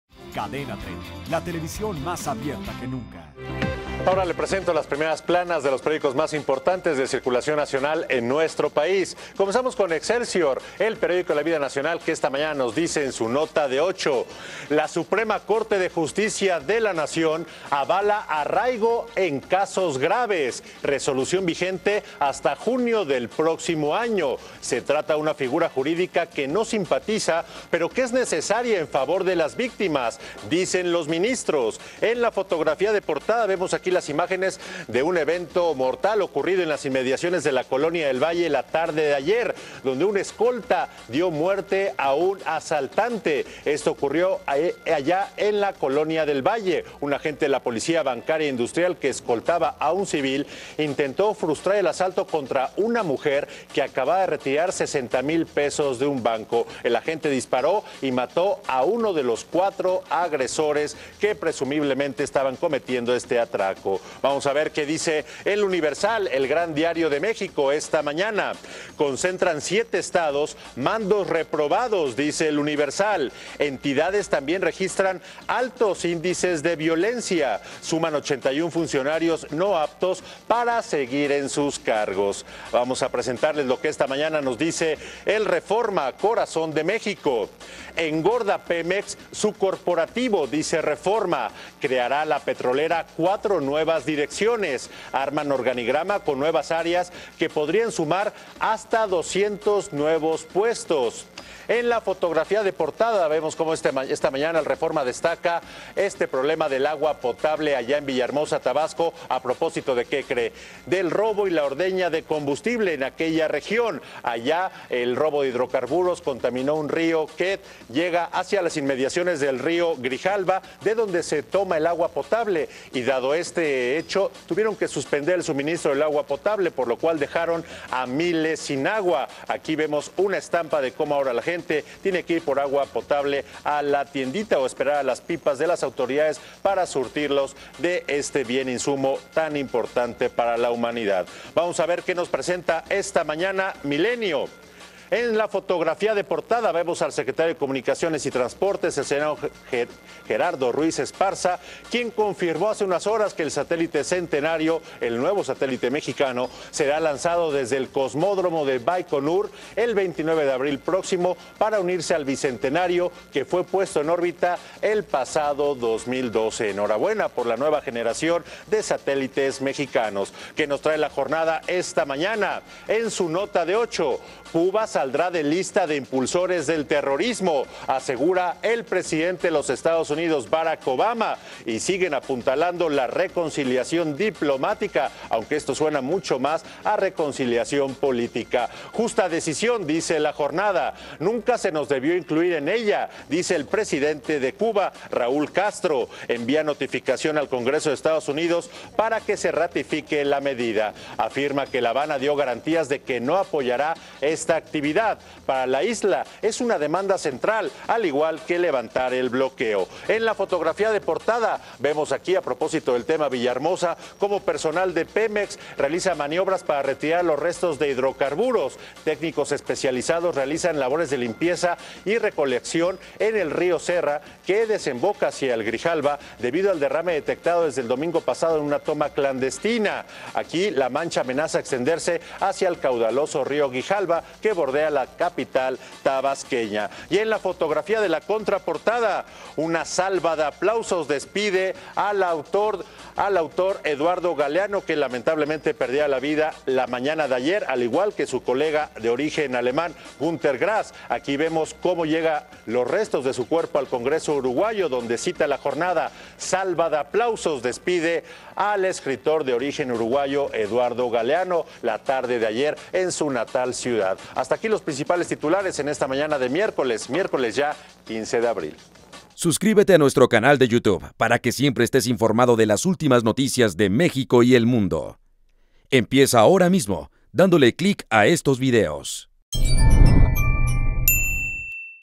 Así amanecieron los periódicos más importantes de México / 15 de abril
Te mostramos como amanece la prensa escrita, las portadas de los periódicos más importantes del país.

15 de abril 2015